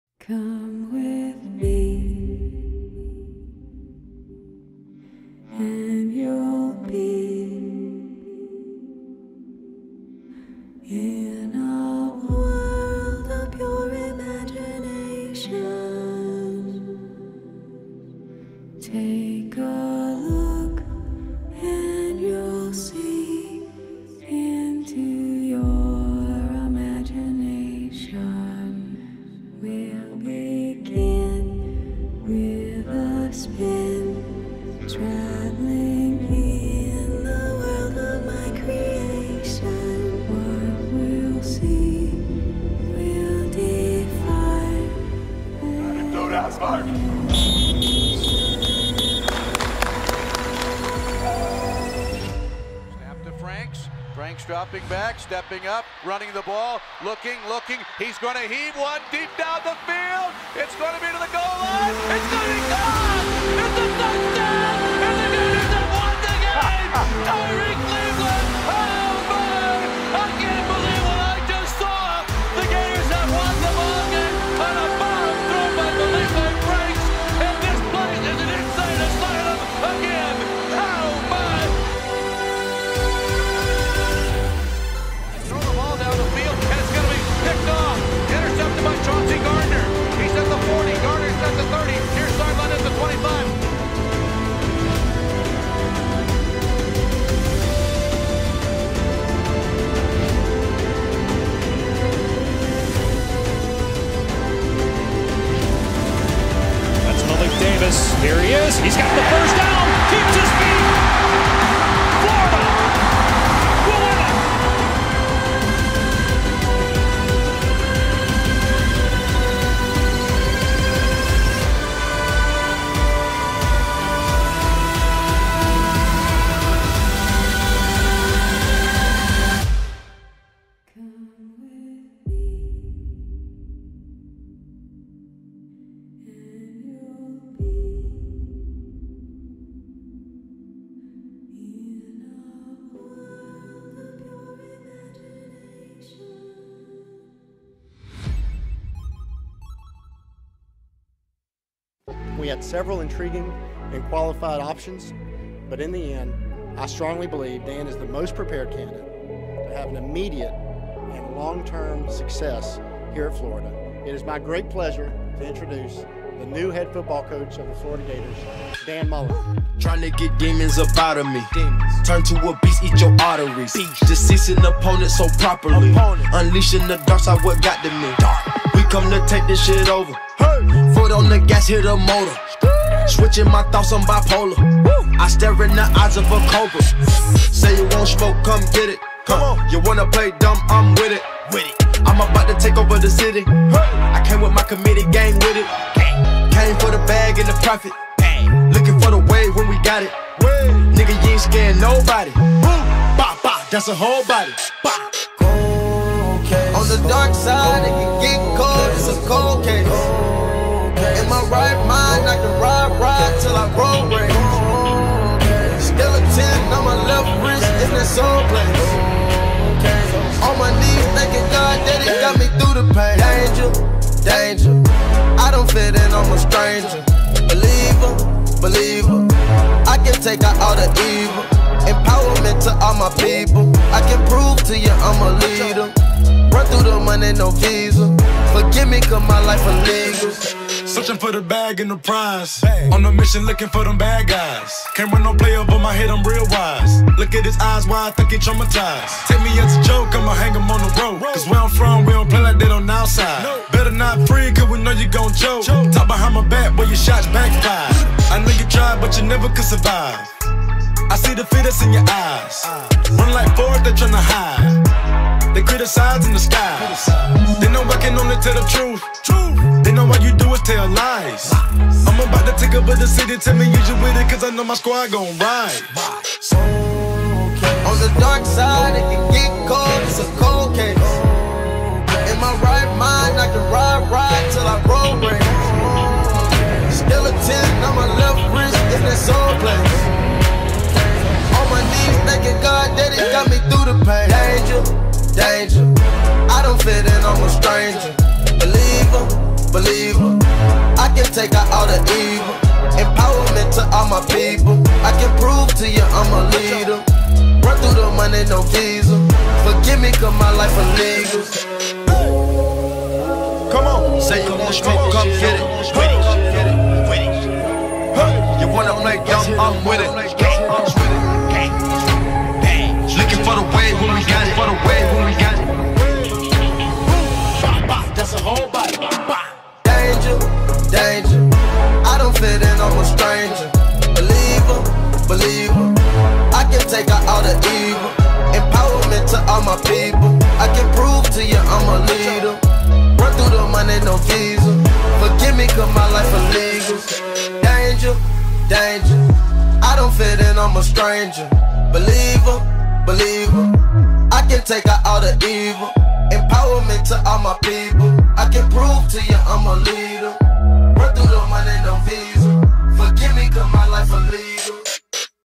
Florida Gators Football - Official 2018 Pump Up [HD]
Jim McElwain is out and Dan Mullen is in. After three years of offensive misery, will the new head coach of the Gators finally fix the quarterback problem? Next season, the Gators have a very young team but will still be incredibly talented across the board. With veterans like Jordan Scarlett and Cece Jefferson returning, you can expect there to be strong leadership. Will the Gators be making a trip to Atlanta next December?